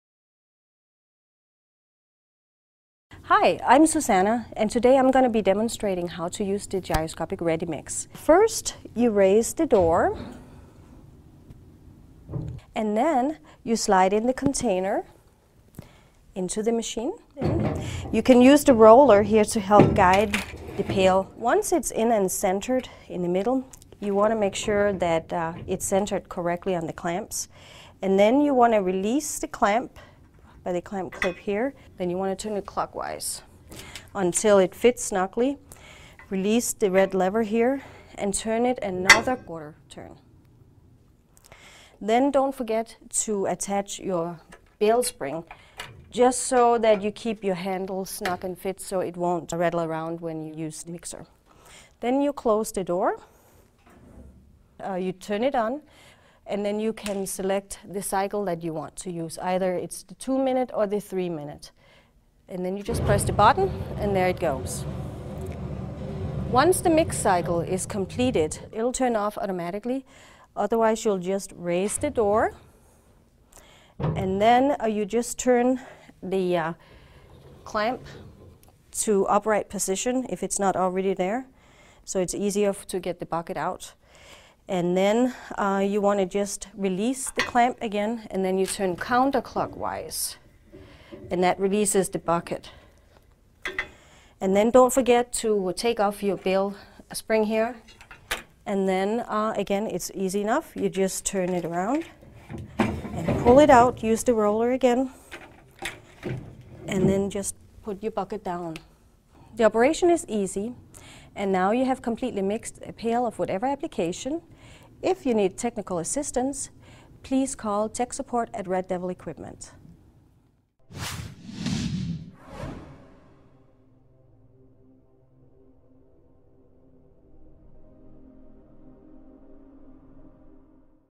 Red Devil Gyroscopic Shaker 5025 - Operation Guide | INDCO
How to operate the 5025 Red Devil 5 gallon gyroscopic shaker (RD-5025-00-PRF) available from INDCO.

INDCO is a leading manufacturer and distributer of industrial mixing equipment.